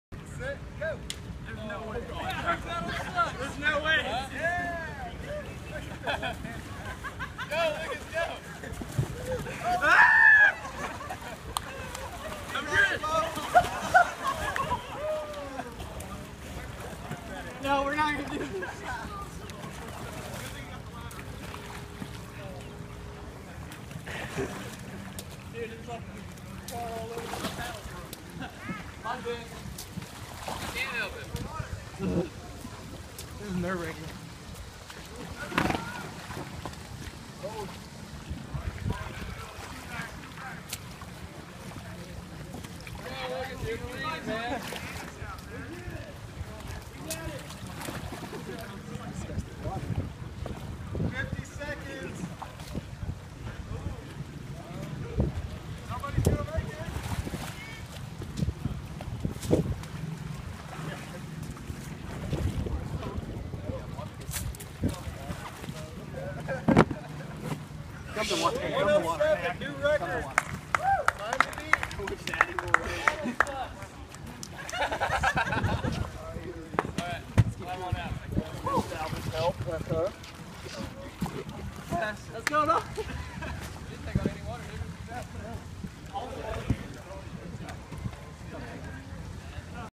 UAFS Freshman Engineering Cardboard Boat race
UAFS Freshman Engineering students race cardboard boats they designed and built. Course taught by Dr. David Paulus.